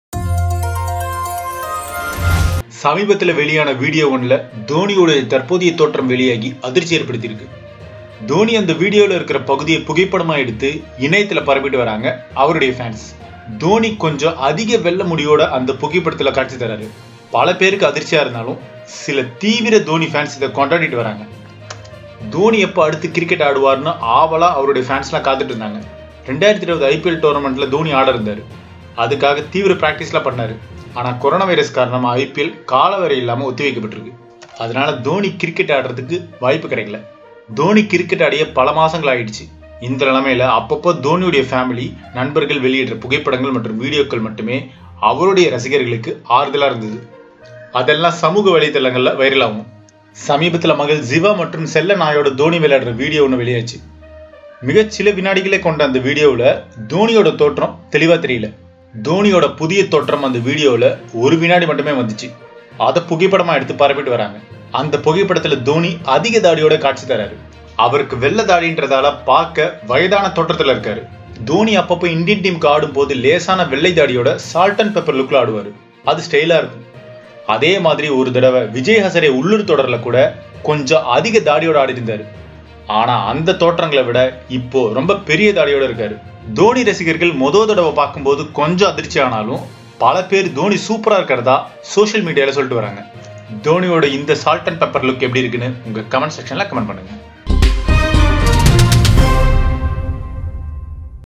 Dhoni’s new white beard look is trending
சமீபத்தில் வெளியான வீடியோ ஒன்றில் தோனியின் தற்போதைய தோற்றம் வெளியாகி அதிர்ச்சியை ஏற்படுத்தி உள்ளது.

Dhoni’s new white beard look is trending in social media. Some fans are shocked while some are enjoying it.